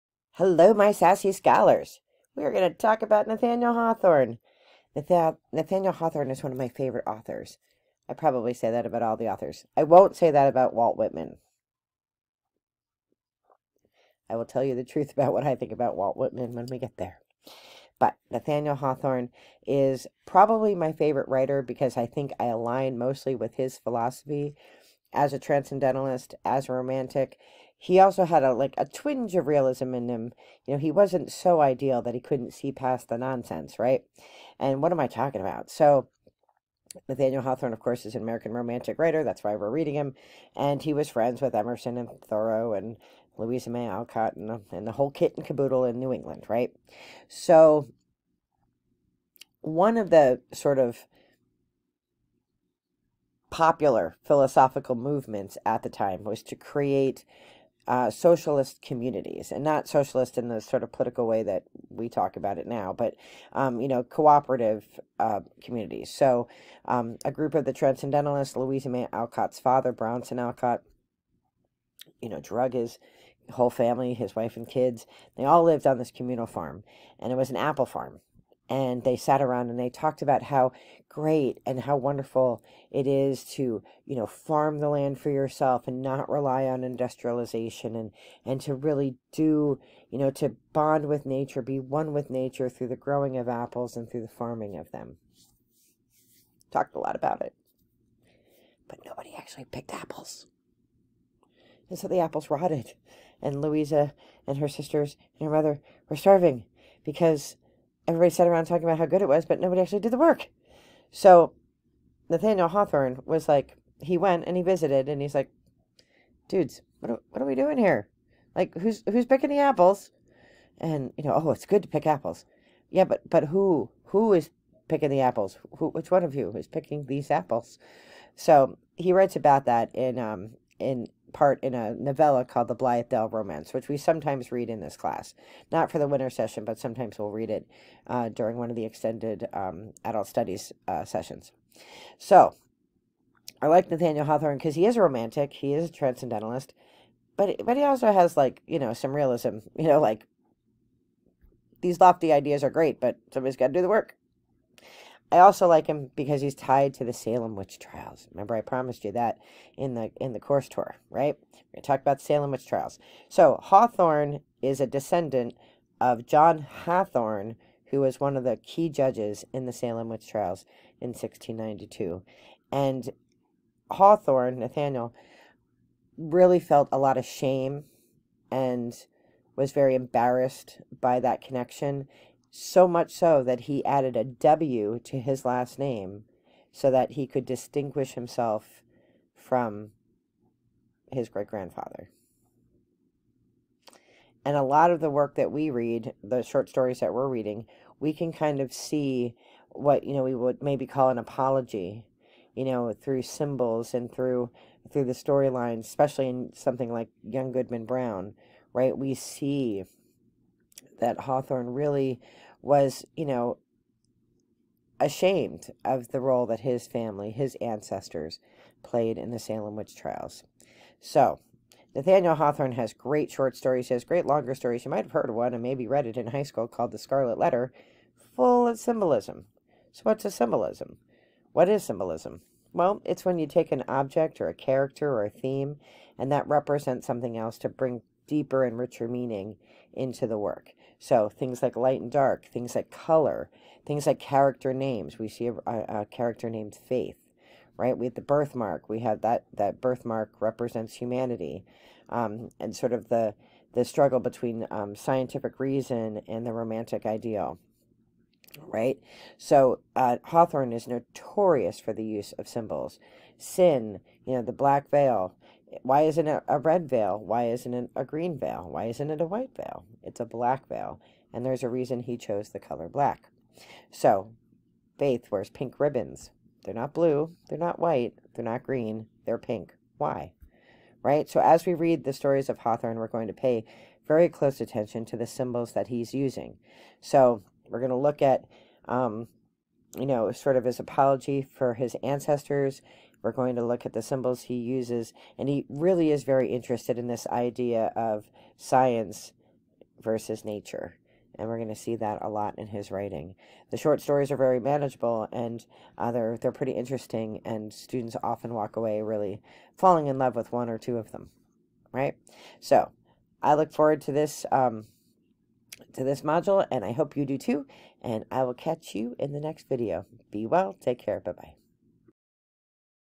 EN250: Nathaniel Hawthorne Lecture Notes
Lecture notes for EN250: American Romantics